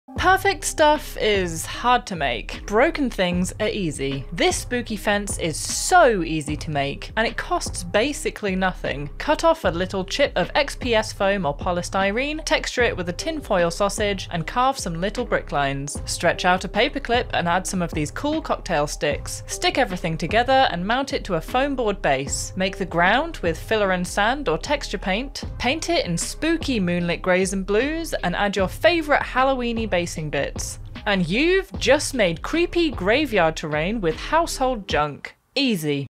How to make a spooky miniature fence
Turn fancy cocktail sticks and a paperclip into an awesome wonky gothic fence!